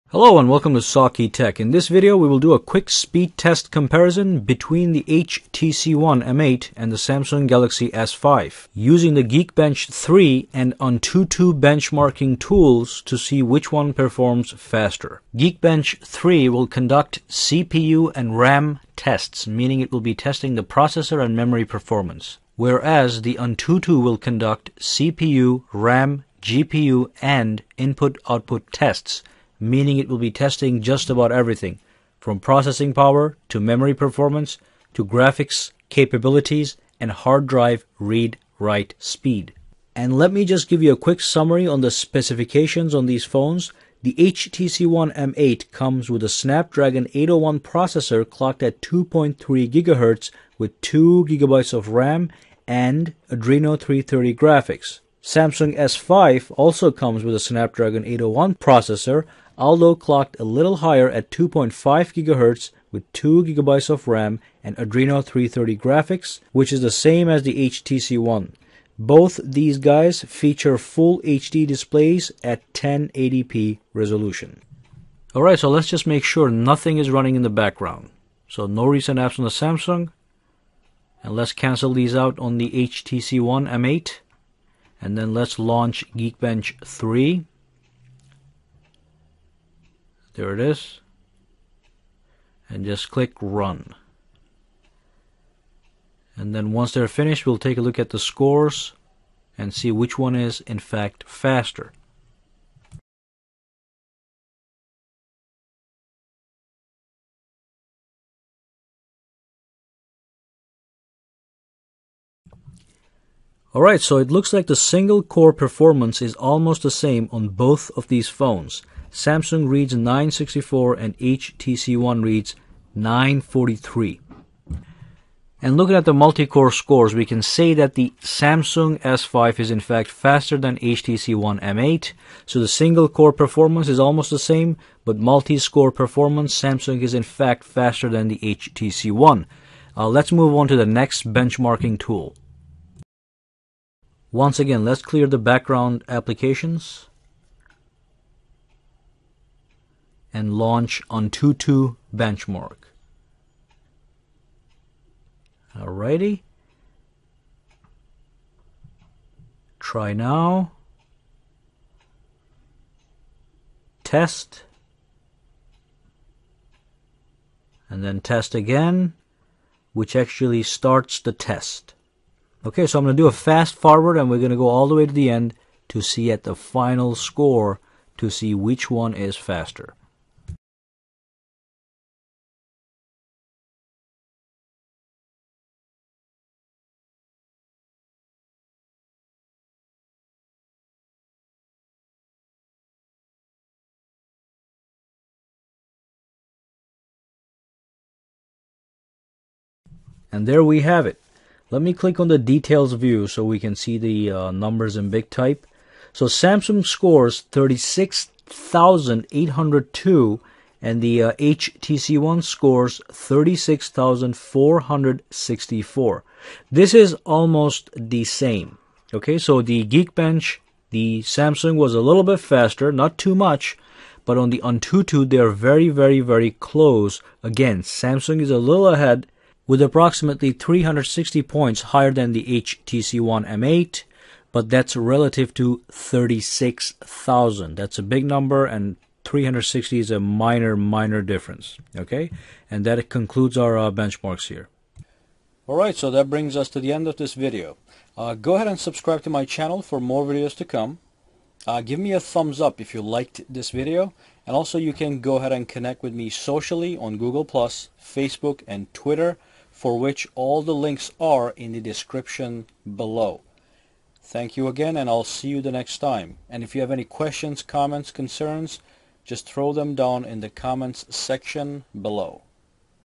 HTC One M8 vs Samsung Galaxy S5 Speed Test Comparison
In this video we will do a speed test comparison between the HTC One M8 vs Samsung Galaxy S5. We will measure the processor, memory, graphics and read write performance using geekbench 3 and antutu benchmakr tools. Enjoy.

FOLLOW ME